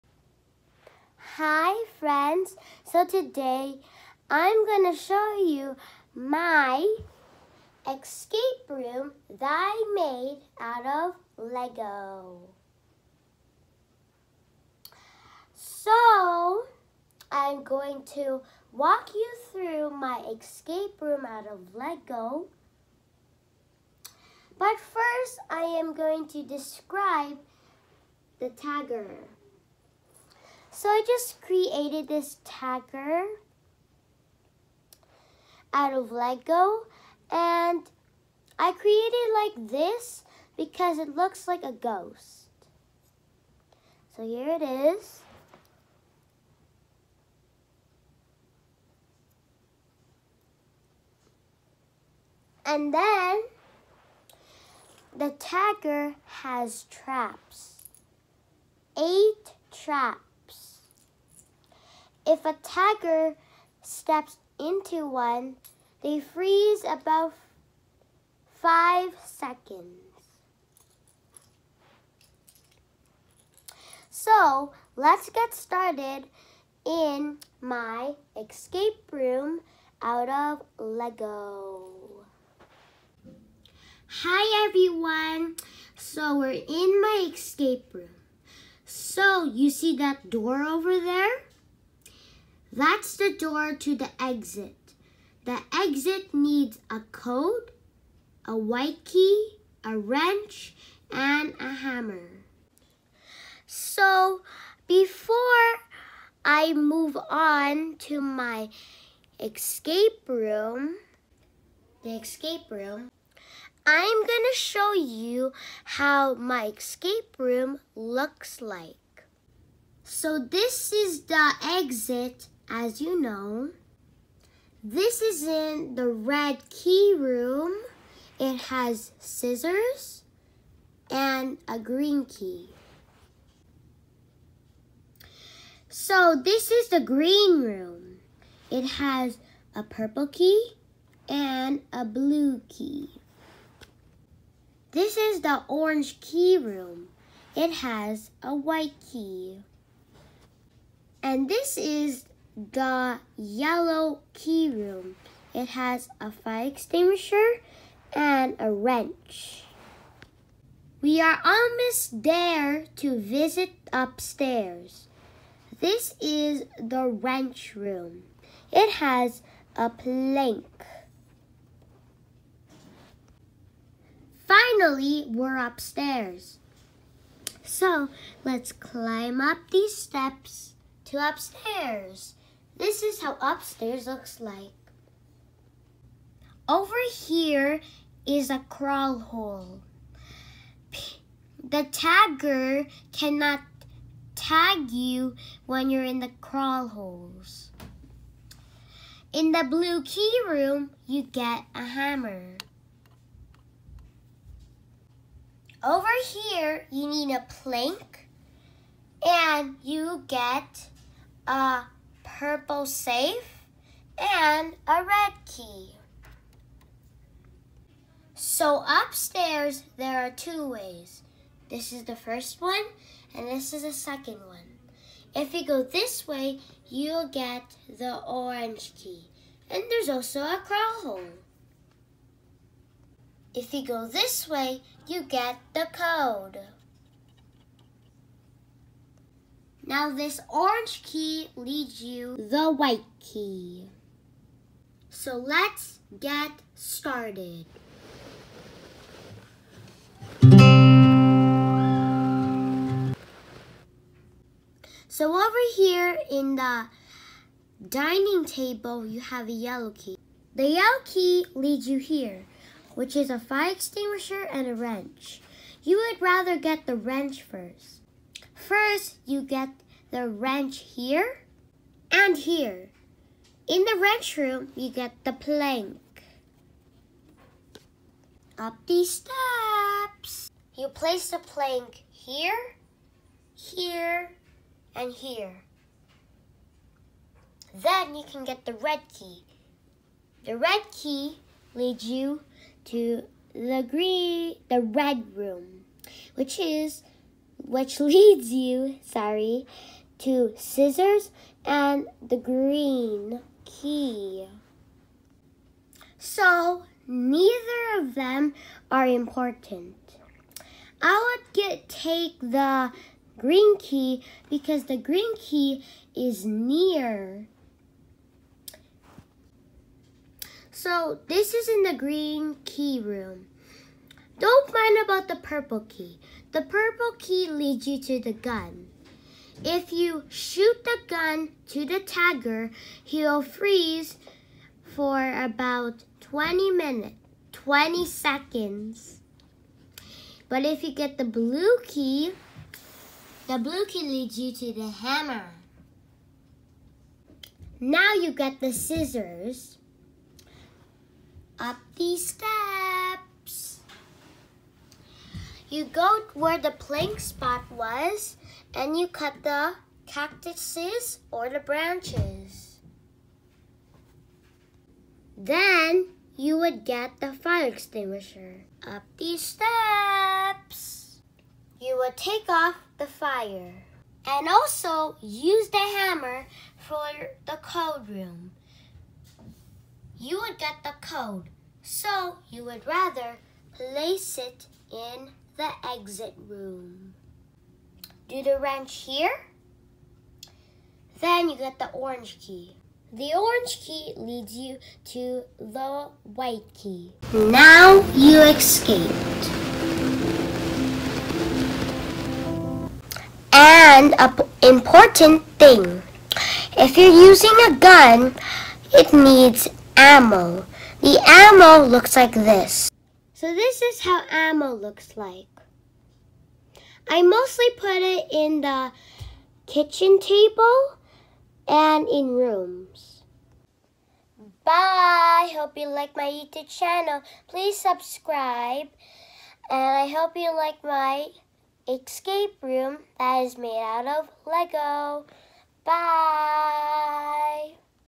Escape room
Enrico builds an escape room out of his lego